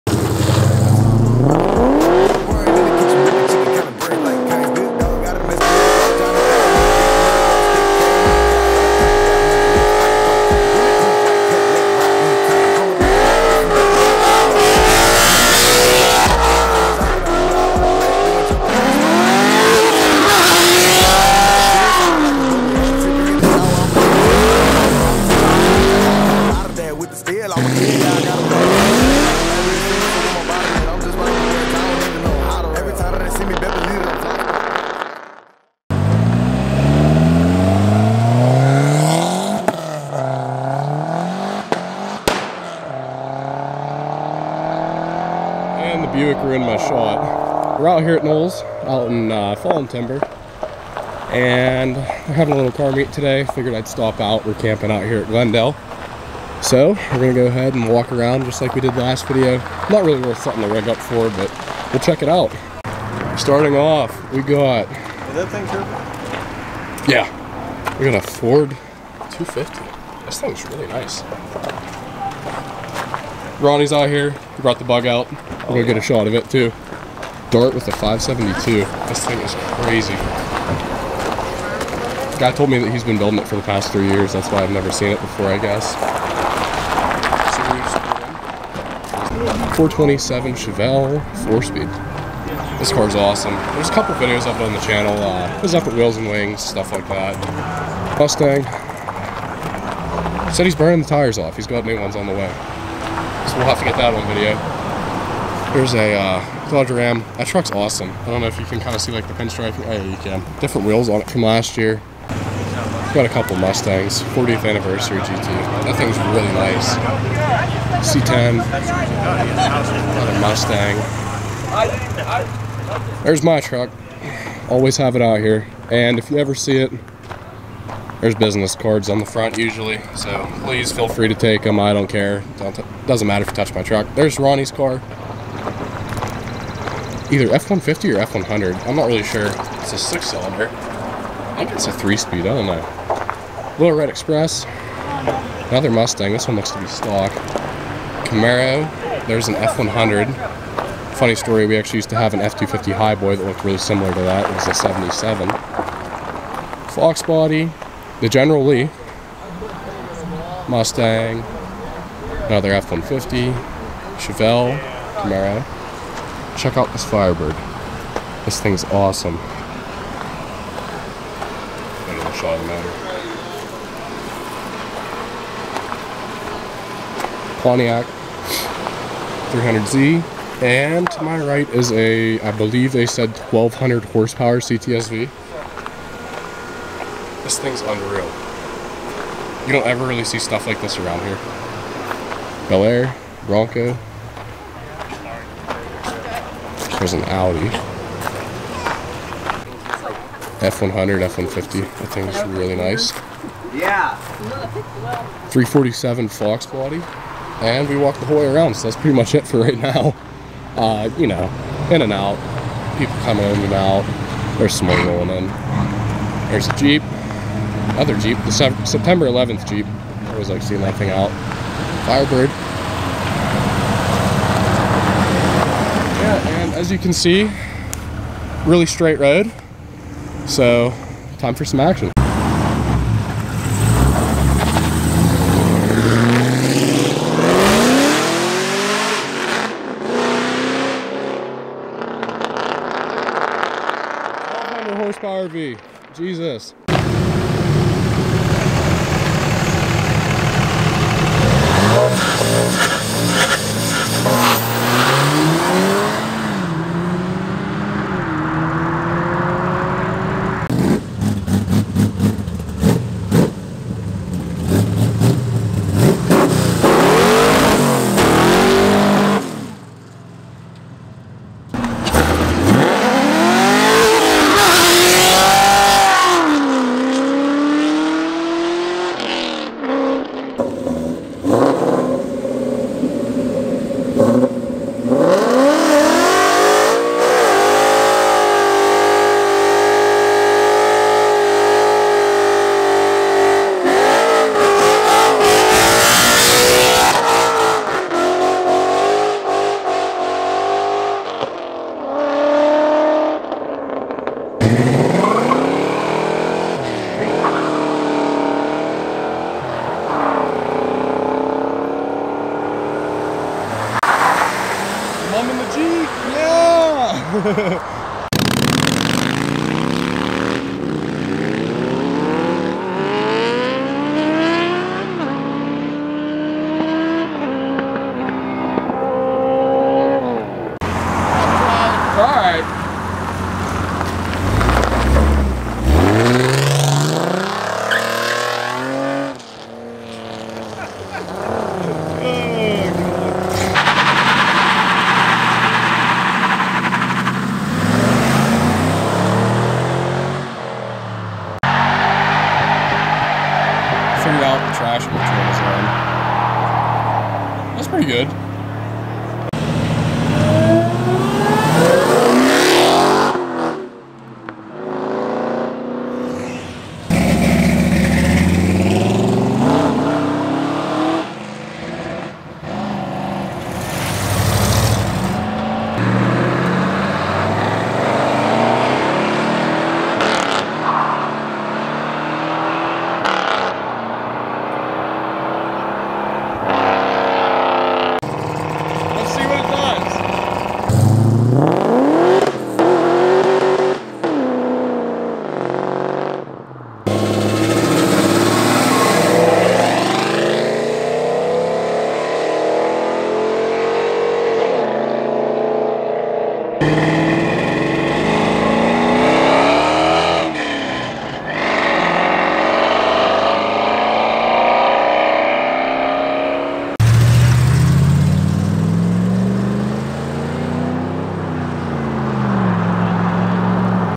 Muscle Cars SEND IT At Memorial Day Car Meet!
In this video I checked out the Noels Car meet! They had a really nice turnout with some awesome rides and some even better sends after the meet!